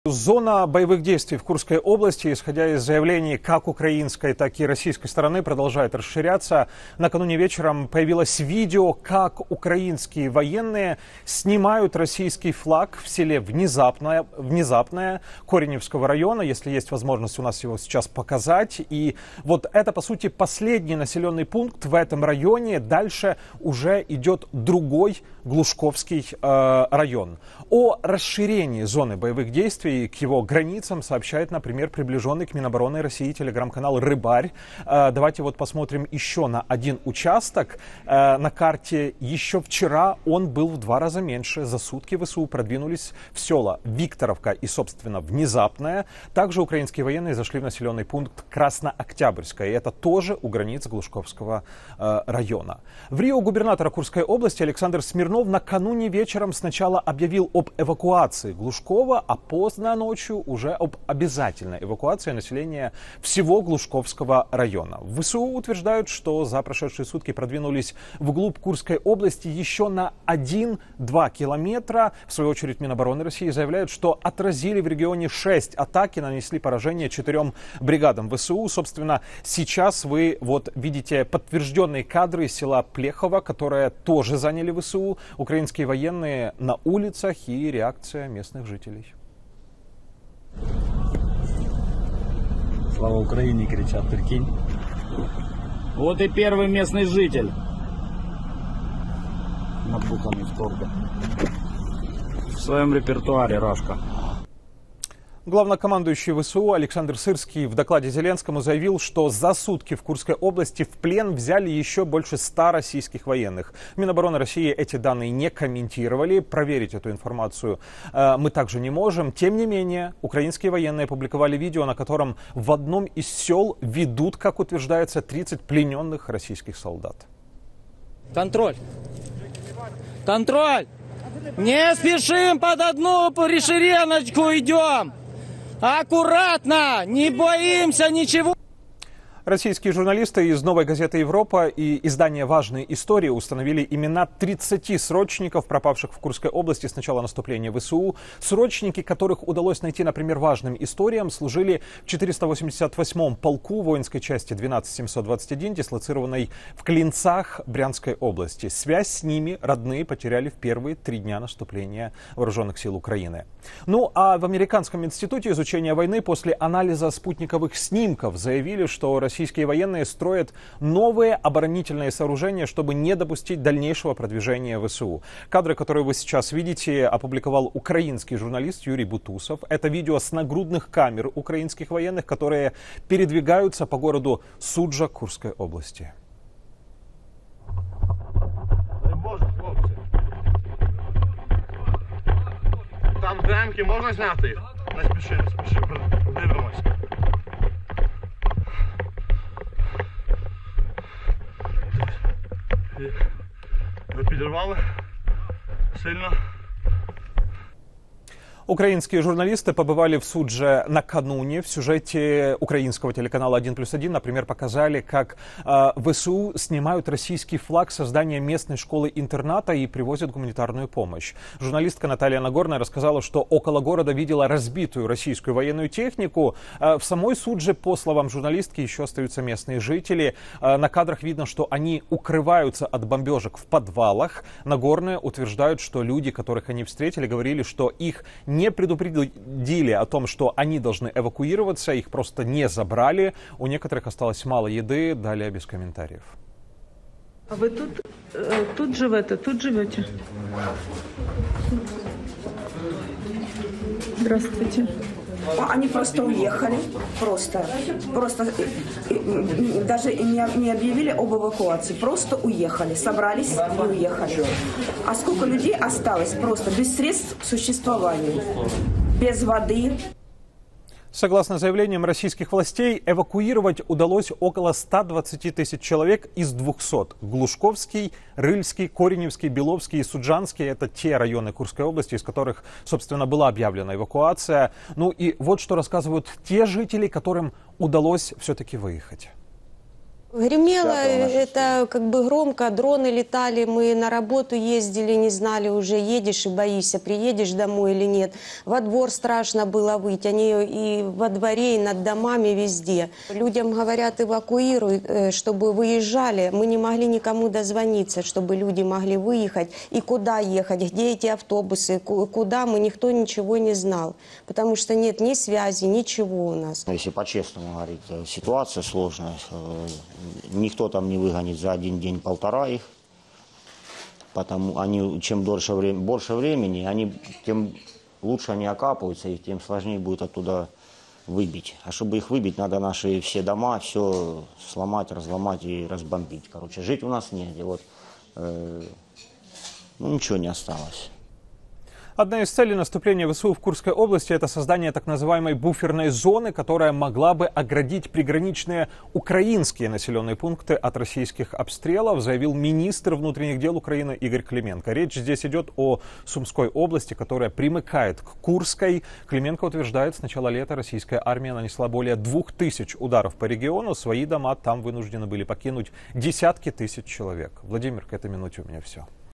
Расширение зоны боев в Курской области: карта атаки ВСУ и последние новости о наступлении
В Курской области идет девятый день наступательной операции ВСУ. Зона боевых действий продолжает расширяться: накануне вечером появилось видео, как украинские военные снимают российский флаг в селе Внезапное — это последний населенный пункт в районе. За сутки ВСУ также продвинулись в села Викторовка и Краснооктябрьское.

Последние подробности наступательной операции ВСУ — в материале Настоящего Времени.